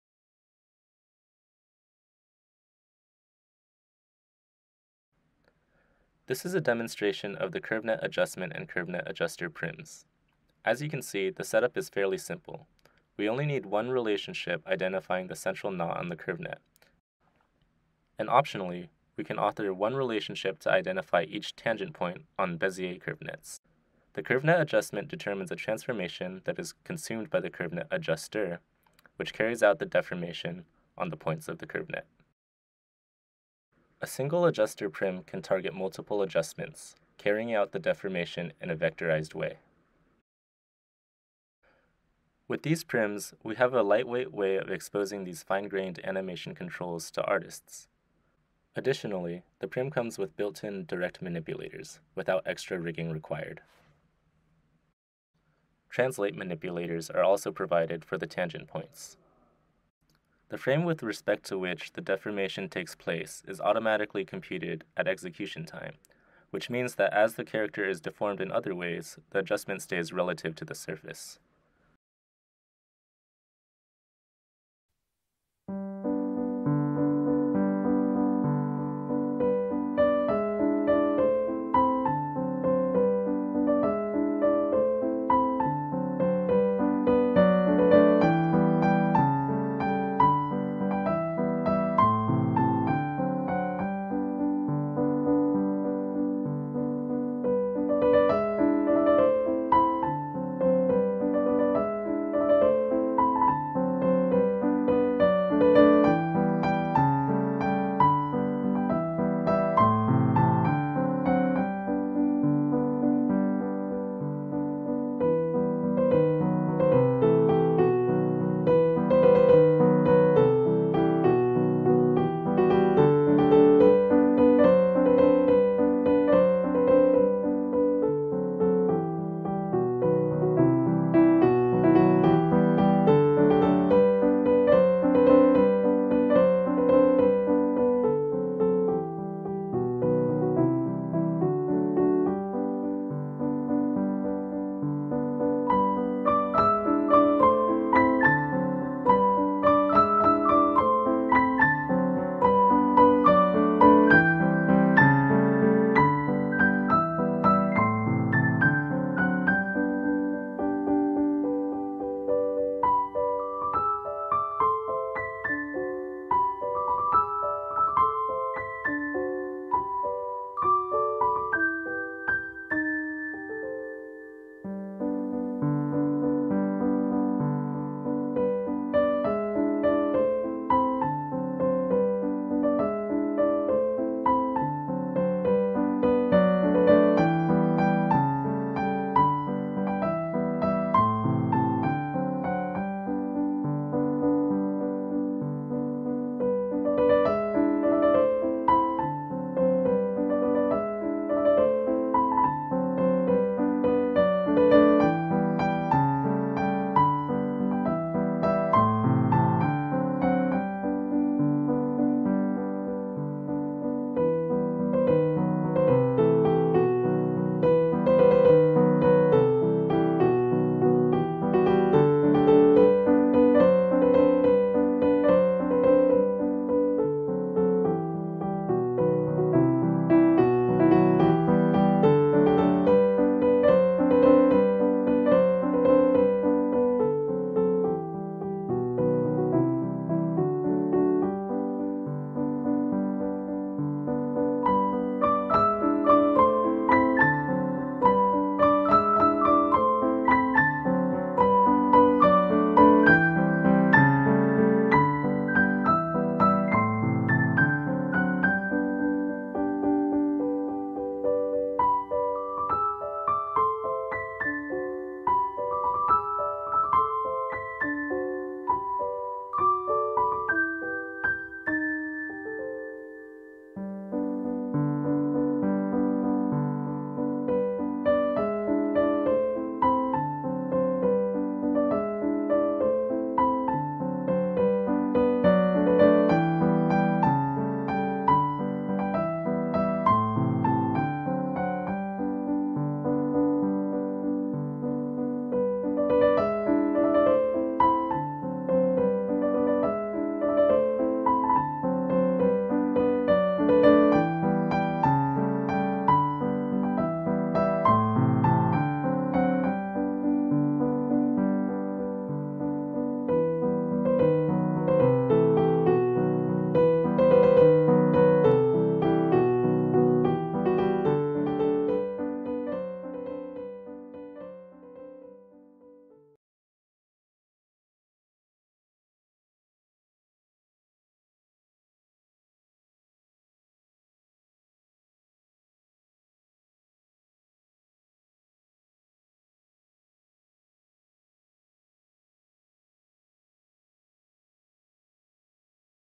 Shaping the Elements: Curvenet Animation Controls in Pixar’s Elemental | 3D Animation Internships
Shaping the Elements: Curvenet Animation Controls in Pixar's Elemental
Nguyen, Talbot, Sheffler, Hessler, Fleischer, de Goes
SIGGRAPH Talks (2023)